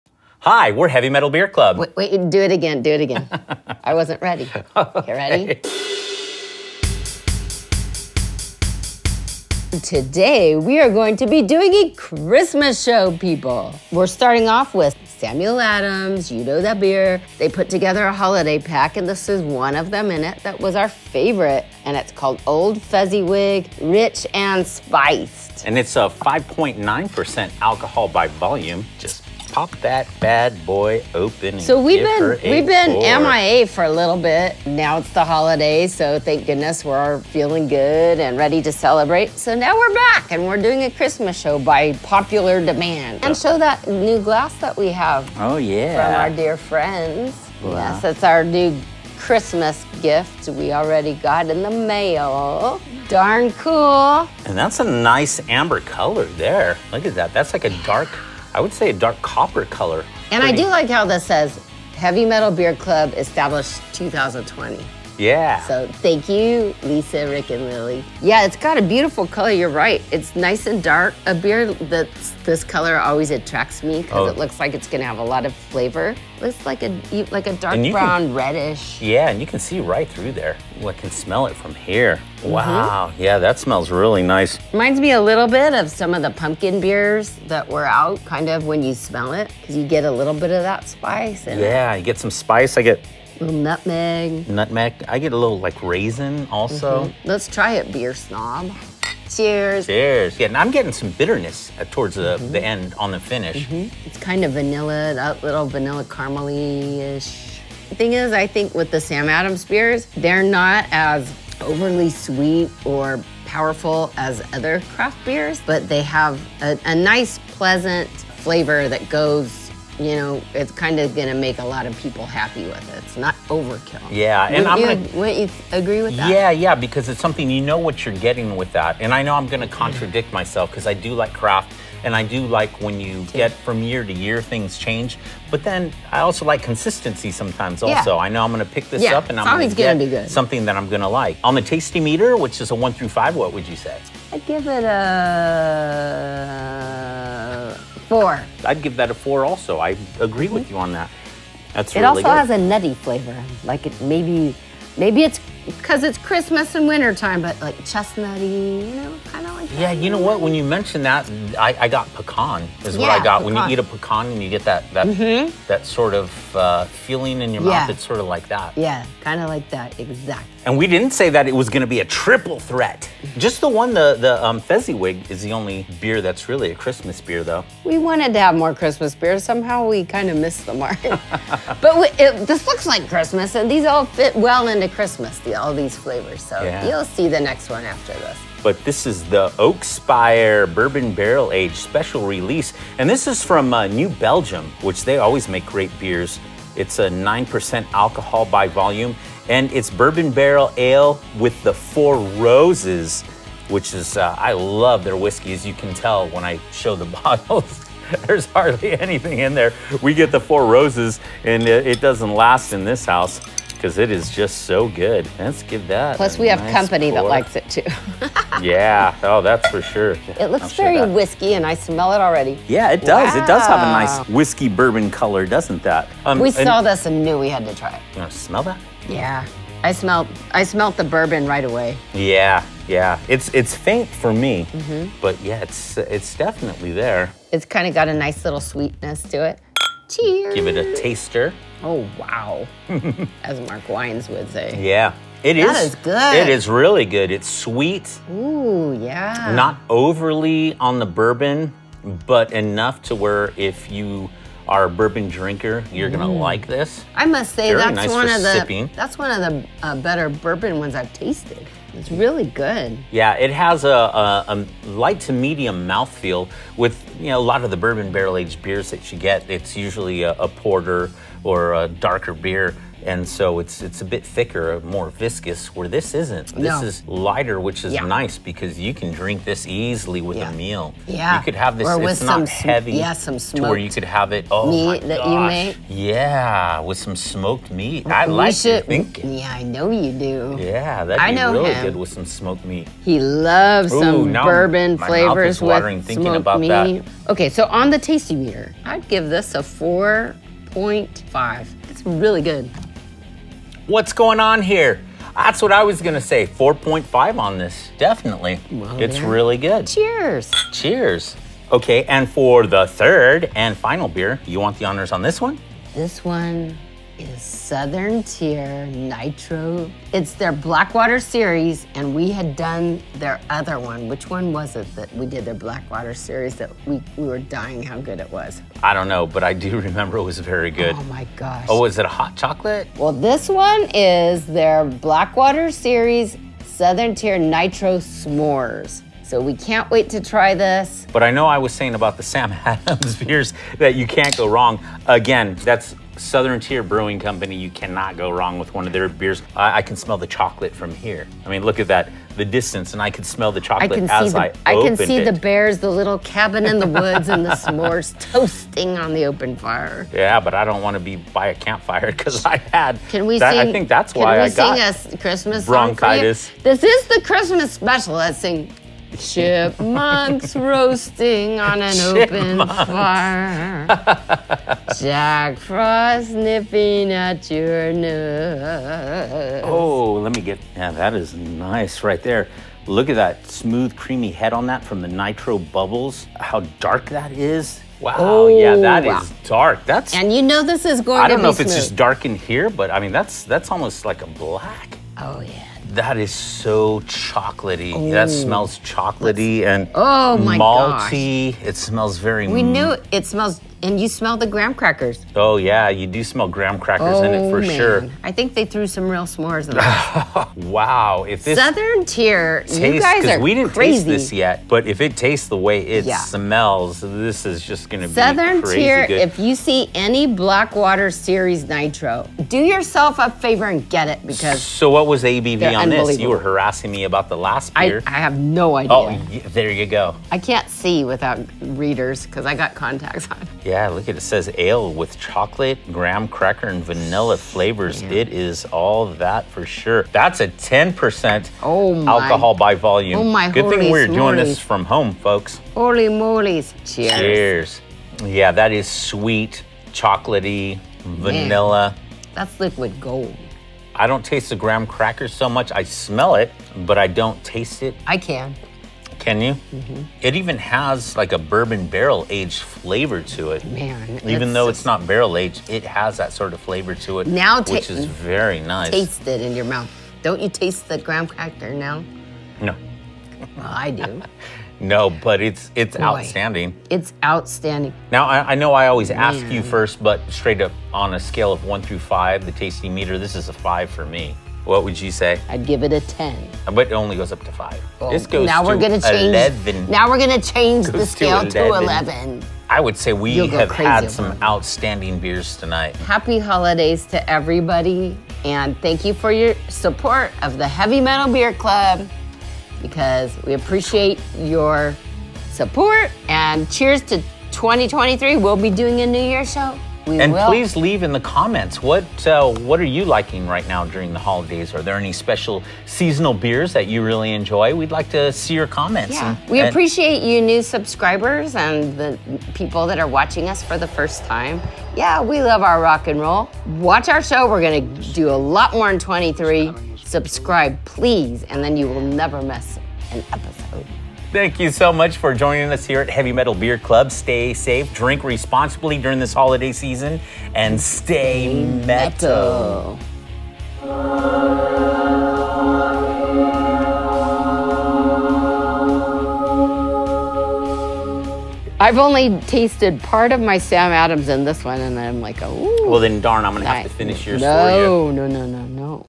HMBC Beer Tasting - Old Fezzywig, Oakspire Bourbon Barrel Ale, and Nitro S'mores Imperial Milk Stout
Heavy Metal Beer Club tastes Samuel Adams Old Fezzywig, New Belgium Brewing Oakspire Bourbon Barrel Ale, and Southern Tier Brewing Company Nitro S'mores Imperial Milk Stout

Gmail @HeavyMetalBeerClub

Songs written and performed by Milton Cox